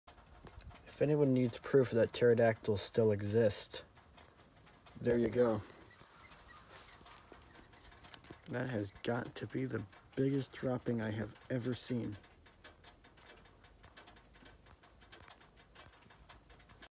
More proof that pterodactyls still exist!
I left the car unattended for about half an hour and I came back to prehistoric crap!